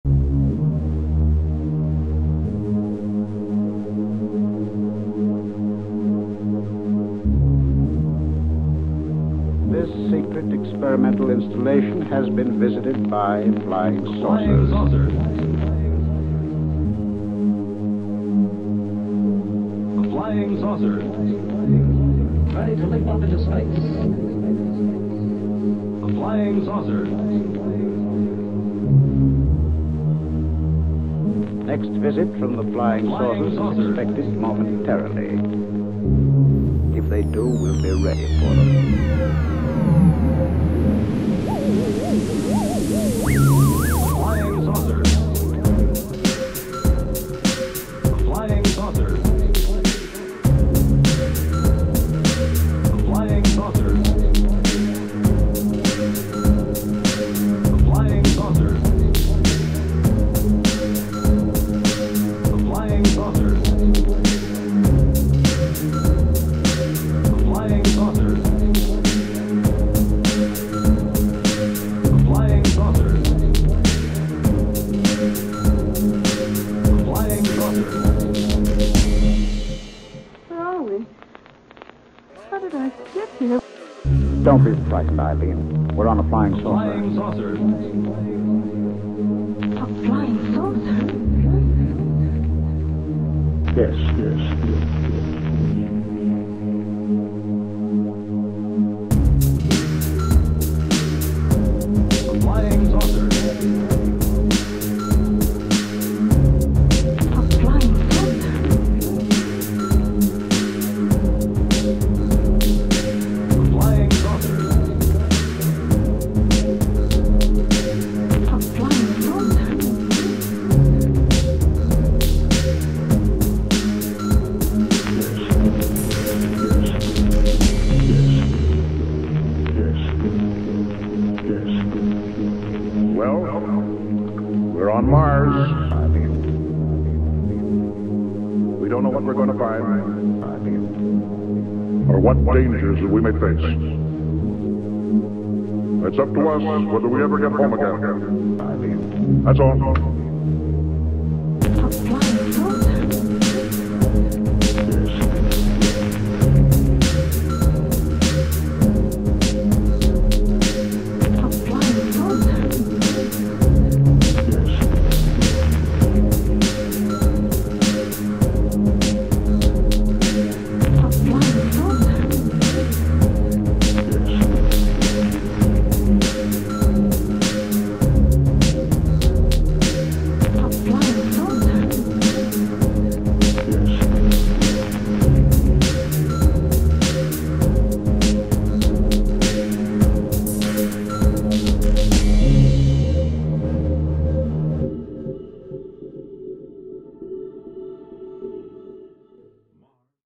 Flying Saucers
Like some (if not all) UAPs this video is transmedium. Clipping together hundreds of shots of UFOS from many varying sources and qualities to painstakingly create a video collage to mystify and intrigue the people of the US and beyond.
A love letter to the phenomena we all know and love or love to hate and fear.
Presented to you, Flying Saucers

From the UAPEP